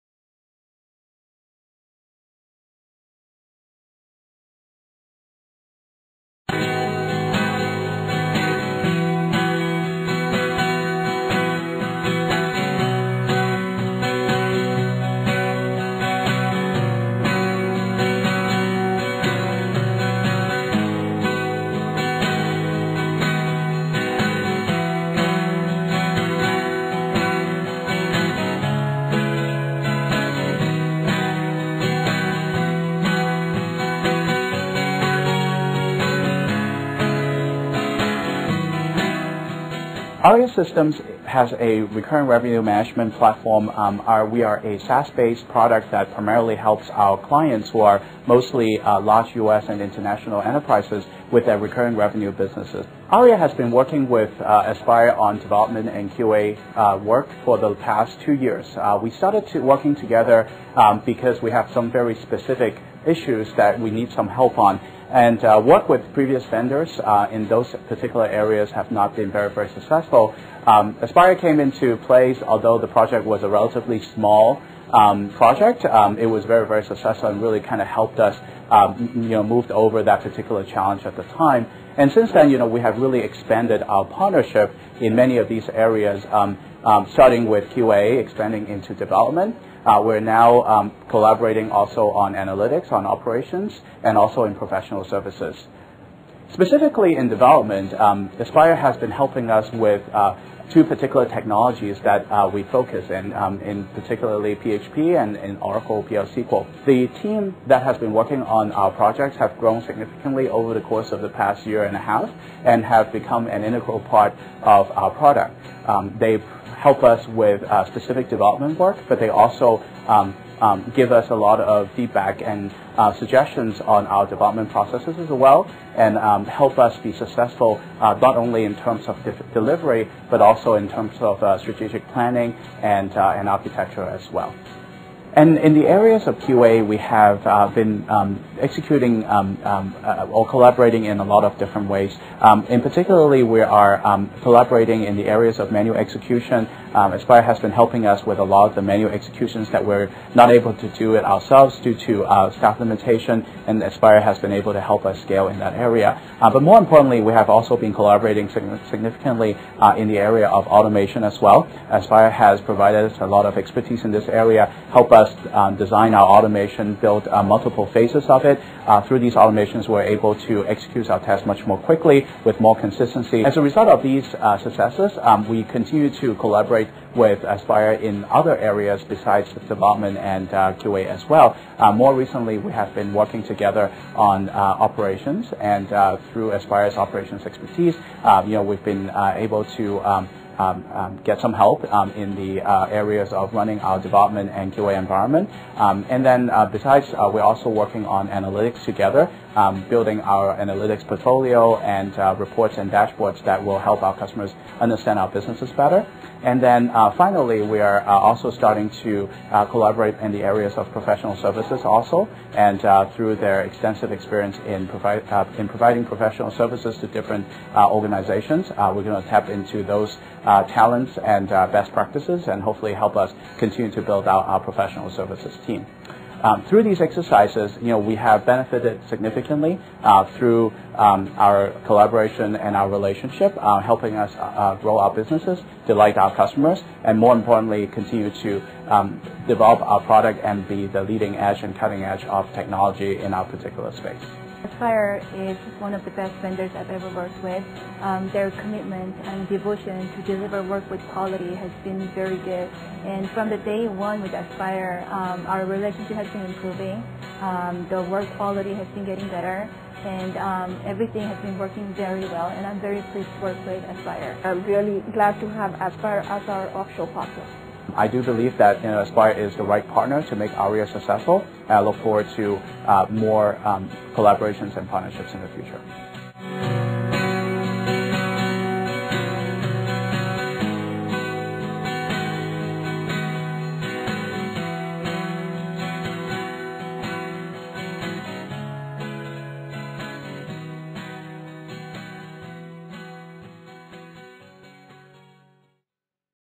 Aria Systems - Testimony
A testimony to Aspire Systems Corporate Philosophy "Attention. Always".